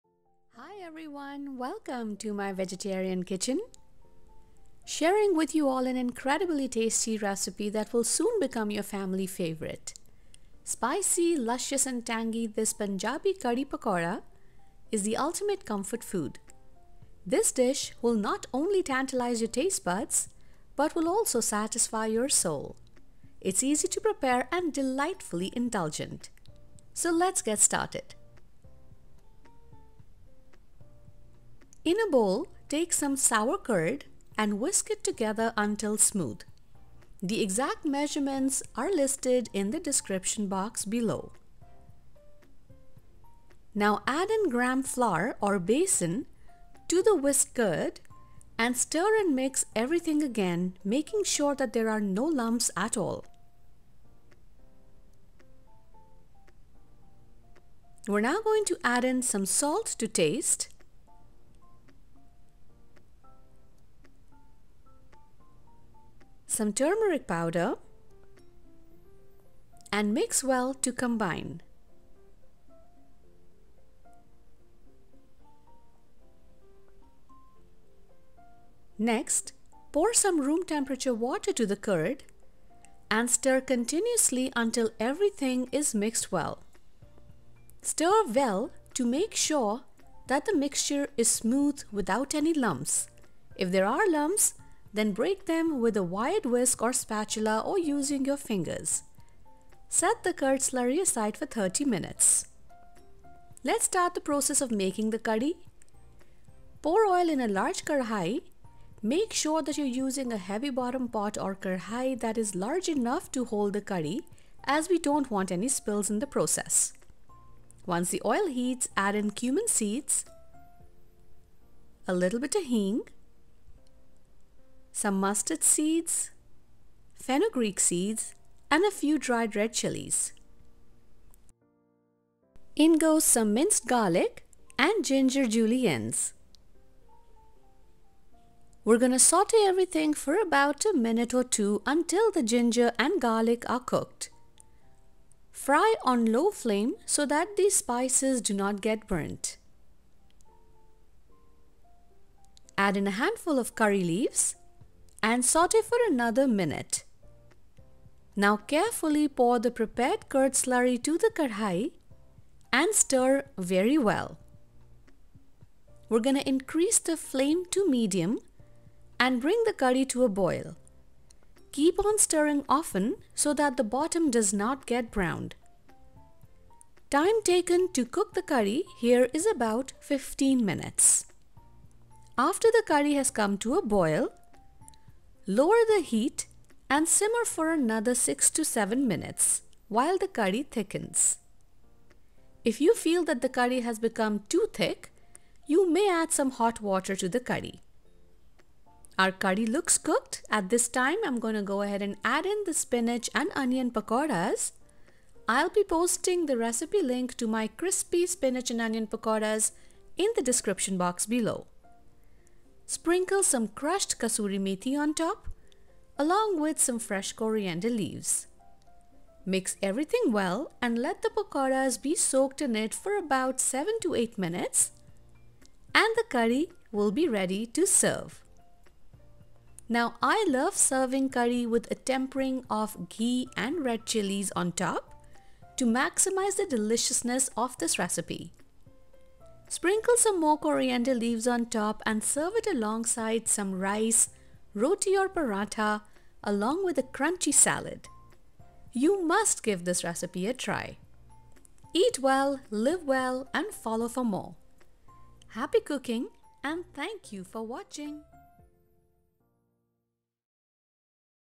Punjabi Kadhi Pakora (My Home Style Recipe)
✨Ingredients:

👉For Curd/Yogurt Slurry:

2 cups sour curd
1/2 cup besan (gram flour)
3 cups of water (or add as required)
11/2 tsp salt
2 tsp turmeric powder

👉For the preparation of Kadhi:

1 tbsp oil
1 tsp cumin seeds
A generous pinch of hing (asafoetida)
11/2 tsp mustard seeds
1 tsp fenugreek seeds
2-3 dried red chillies
3-4 garlic, minced
1 inch piece of ginger, julienned
Handful of curry leaves, chopped (approximately 8-10 curry leaves)

✨Instructions:

Take the sour curd in a bowl and whisk tigether until smooth. Now add the fram flour or besan to the whisked curd, stir and mix everything again making sure that there are no lumps at all. Add salt and turmeric powder and mix well to combine.
Next pour water to the curr and stir continuously until everything is mixed well and set it aside for 30 mins.

To prepare the kadhi, pour some oil to a large kadai or a heavy bottom pan. Once heated add cumin seeds, hing, mustard seeds , fenugreek seeds, a few dried red chillies, garlic and ginger and cook on low flame for a minute or two.
Now add curry leaves and saute for another minute. Carefully pour the prepared curd slurry and stir it very well.

Increase the flame to medium and bring the kadhi to a boil. Keep on stirring often so that the bottom does not get browned (approximately 15 minutes).
After the kadhi comes to a boil, lower the heat and simmer for another 6-7 minutes to allow the kadhi to thicken.

Now add the prepared spinach and onion pakoras, sprinkle some kasoori methi and fresh coriander leaves on top and mix well . Let the pakoras be soaked in it for 7-8 minutes and it’s ready to serve.

Pour a ghee and red chillie powder tempering on top maximize the deliciousness of the recipe. Sprinkle some fresh coriander leaves on top and serve with rice, roti or parantha along with a crunchy salad.
Link to get the recipe for my Crispy Spinach & Onion Pakoras

Happy Cooking 🧑‍🍳